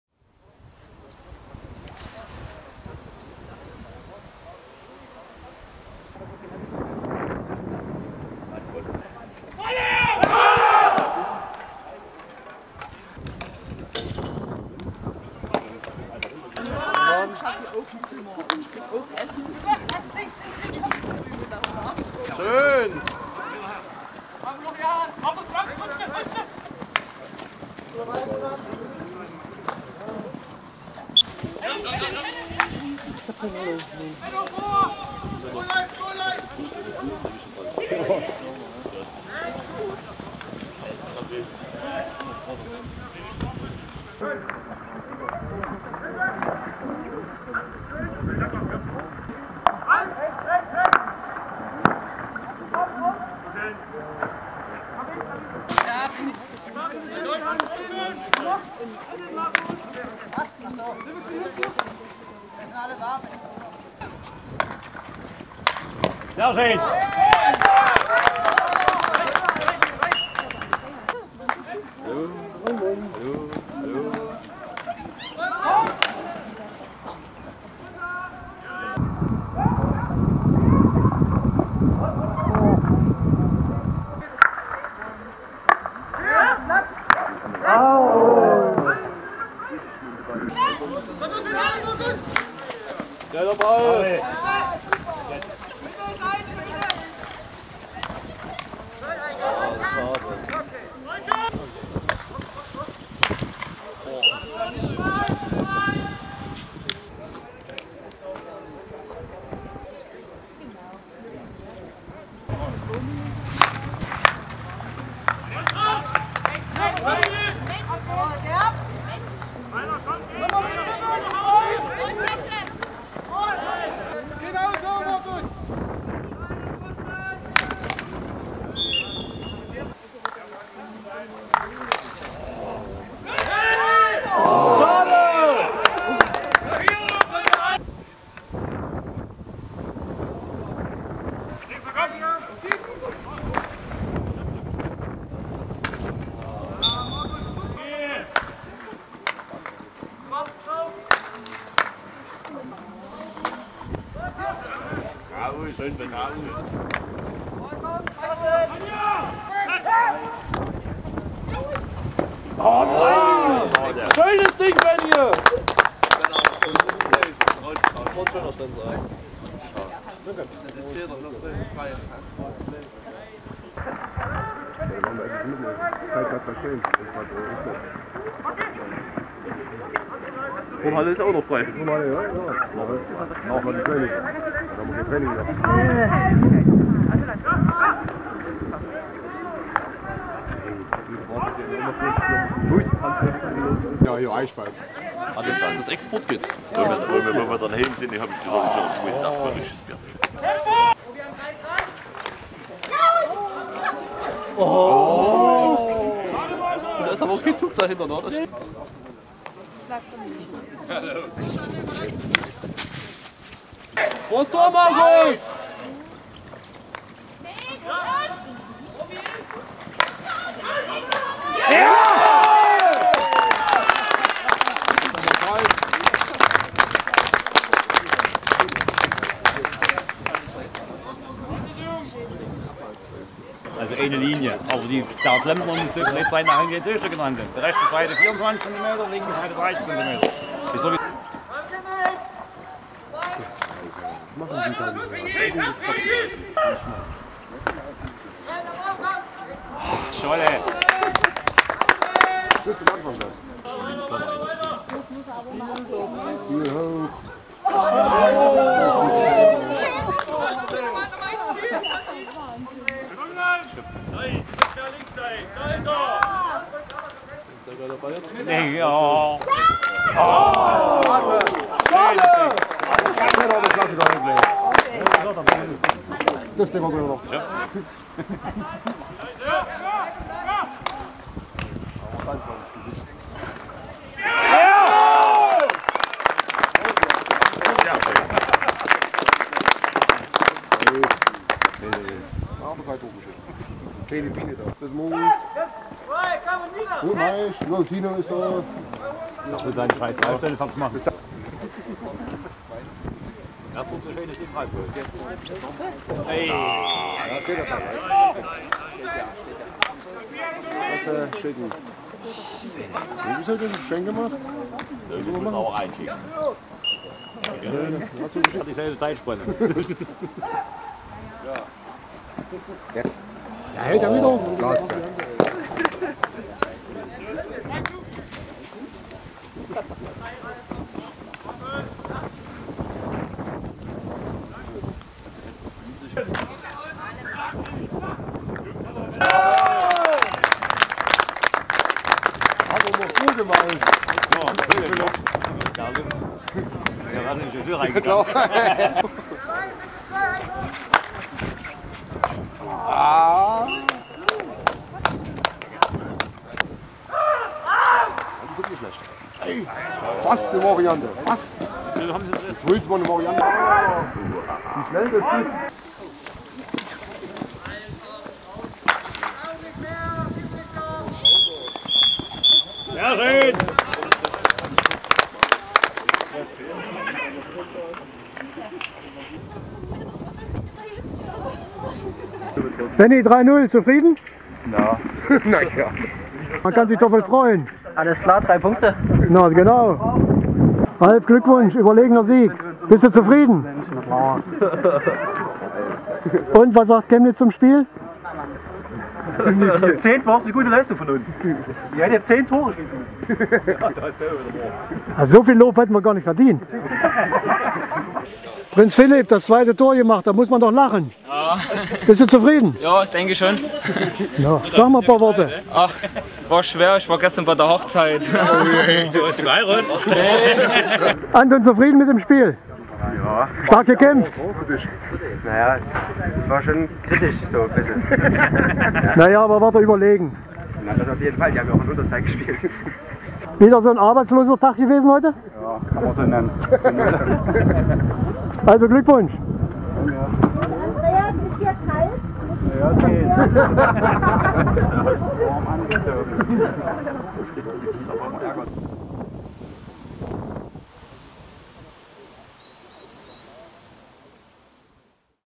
Freiberg _Hockey _FHTC - PSV Chemnitz _20.09.2015
Schwer zu überwinden war die Abwehr und rein gar nicht der überragende Torwart vom PSV Chemnitz in der ersten Spielhälfte. Dann aber, weil personell und konditionell geschwächt, konnte auch der PSV Rückhalt Tore nicht mehr verhindern. Freiberg gewinnt drei Punkte.